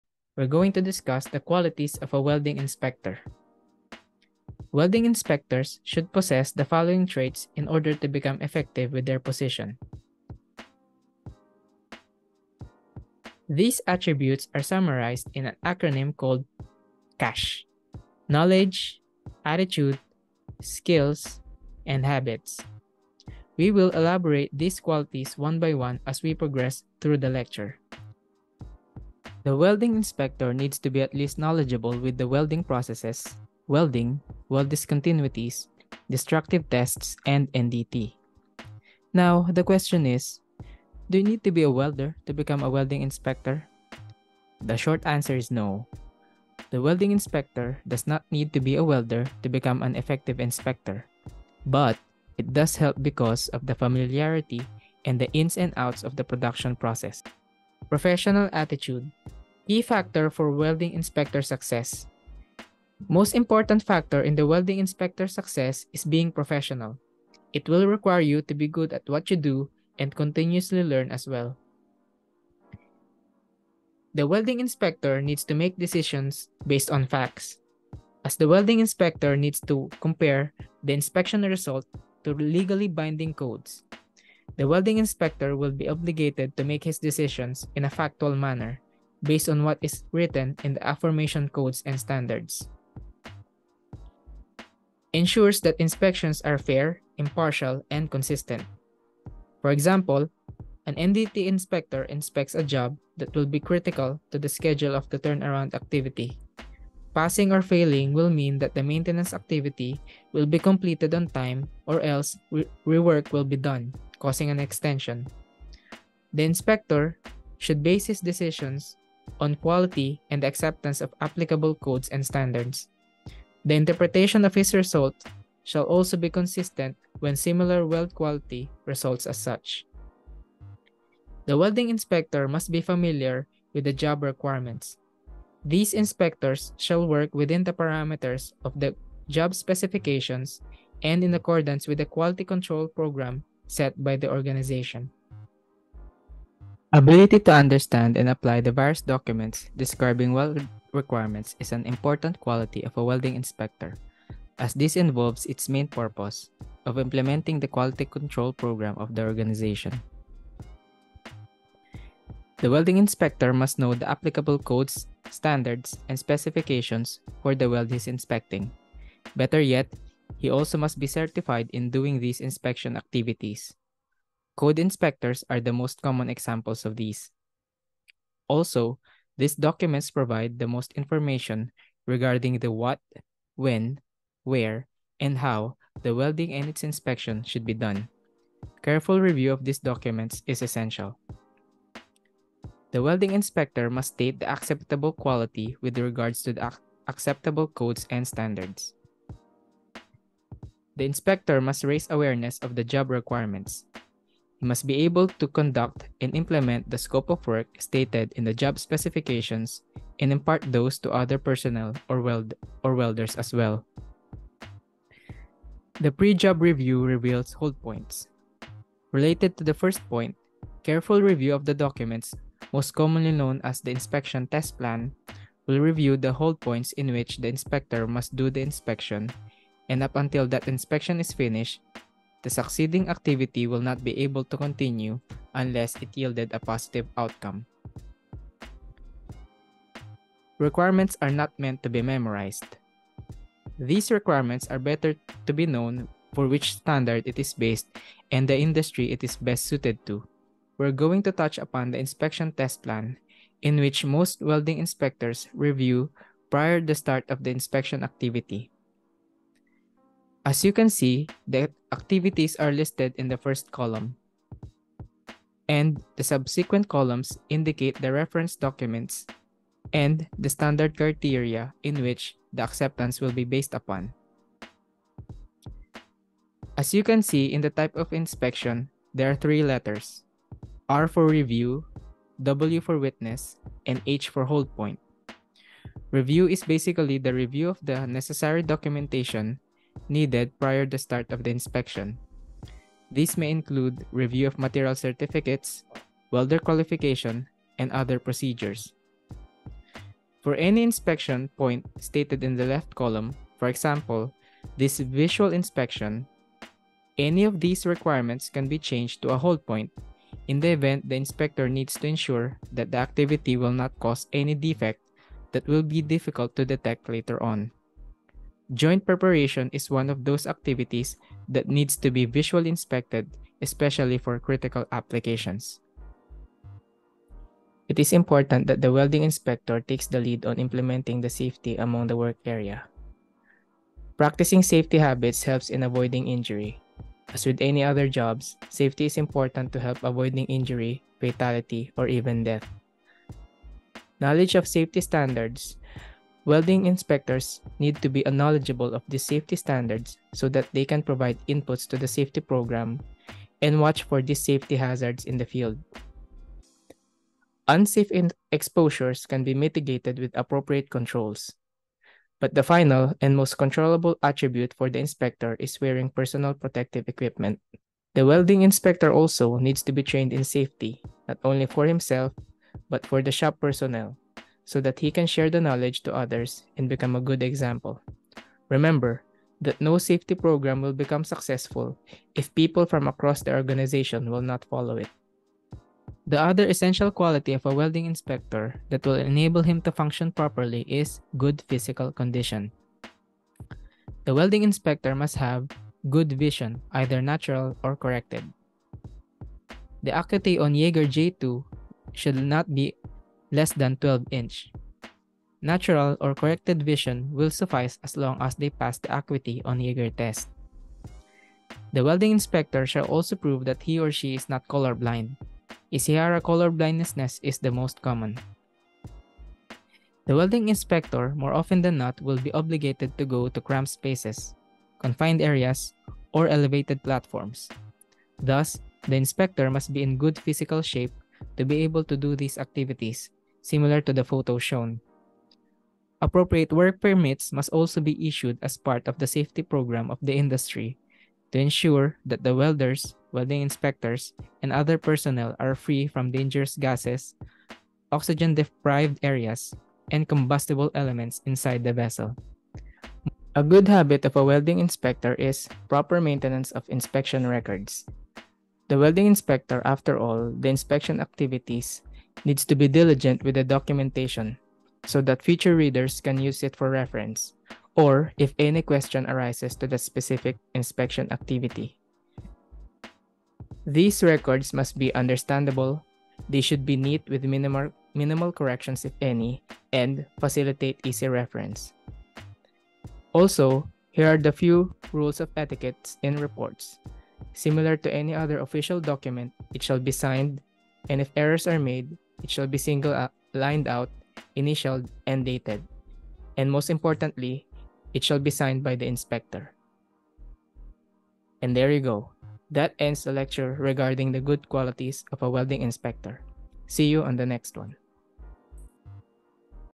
Certified Welding Inspector Course: Qualities of a Good Welding Inspector
Welcome to this Certified Welding Inspector (CWI) course video, where we delve deep into the qualities that make a good welding inspector stand out in the field. Whether you're aiming to become a CWI or seeking to enhance your skills as an inspector, this video is designed to provide you with invaluable insights and practical knowledge.

In this course, we explore the essential traits that every successful welding inspector should possess. From the acronym KASH, Knowledge, Ability, Skills and Habits, this video elaborates the necessary traits and qualities that every welding inspector needs to possess.